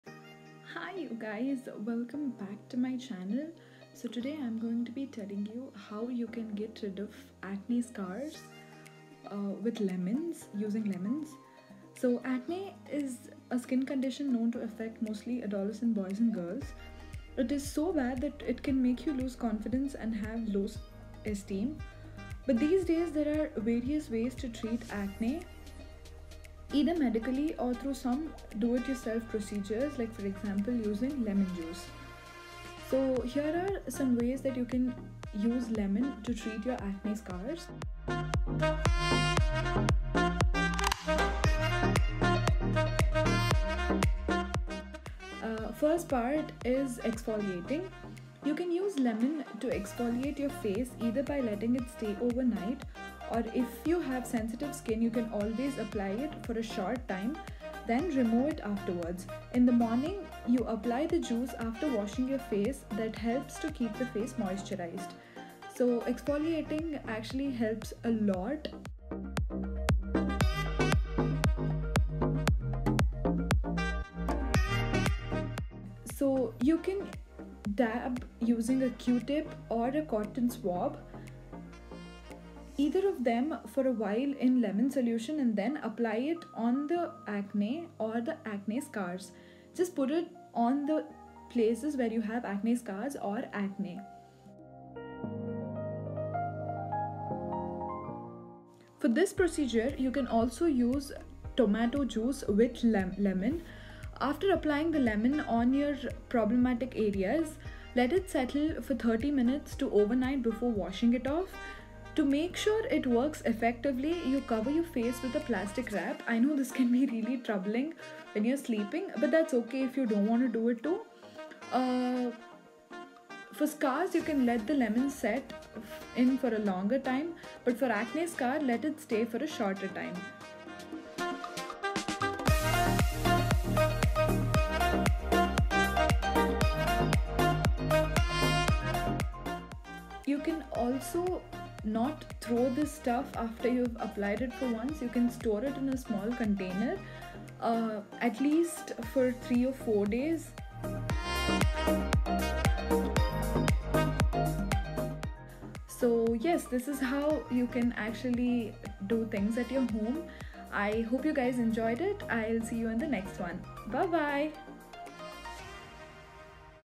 NATURALLY GET RID OF ACNE SCARS using LEMON! How to use Lemon for Scarring and Dark Spots
Hey guys!
This video is about how to naturally get rid of acne scars and scarring using lemon!
It’s an easy homemade acne scar remedy anyone can use. And this is good for dark spots too!